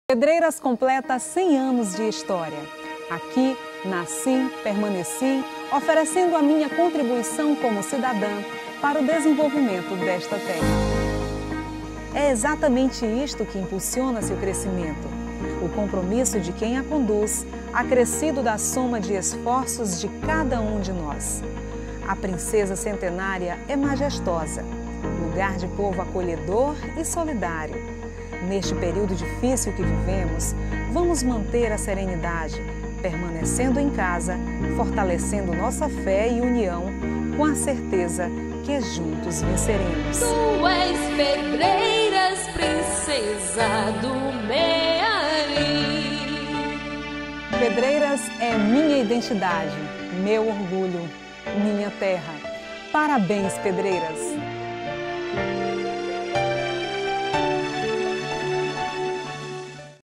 Homenagem de Katyane Leite aos 100 anos de Pedreiras
Em uma mensagem de otimismo e amor a Pedreiras, a comunicadora e educadora, Katyane Leite, expressa seu mais profundo sentimento de alguém que nasceu, cresceu e viveu nesta terra e conclama união de esforços de todos para a construção de melhores dias.

A princesa majestosa – em que enaltece Pedreiras, chama a atenção para a valorização do que é sua terra; seu povo acolhedor e solidário – é a sua própria essência enquanto filantropa, altruísta e humanista, emergindo e se apresentando com a força de quem quer olhar para a frente com espírito de luta, não querendo emblemar seu discurso em tempos difíceis, mas de esperança, fé e união, certa da vitória, que é de todos os pedreirenses.
Katyane Leite:  "Pedreiras completa 100 anos de história, aqui nasci e permaneci, oferecendo a minha contribuição como cidadã para o desenvolvimento desta terra. E é exatamente isso que impulsiona este crescimento, o compromisso de quem a conduz acrescido da soma de esforços de cada um de nós. A princesa centenária é majestosa, lugar de povo acolhedor e solidário. Nesse período difícil que vivemos, vamos manter a serenidade, permanecendo em casa e fortalecendo nossa fé e união, com a certeza que juntos venceremos.

Pedreiras é minha identidade, meu orgulho e minha terra. Parabéns Pedreiras."

Assista no vídeo acima.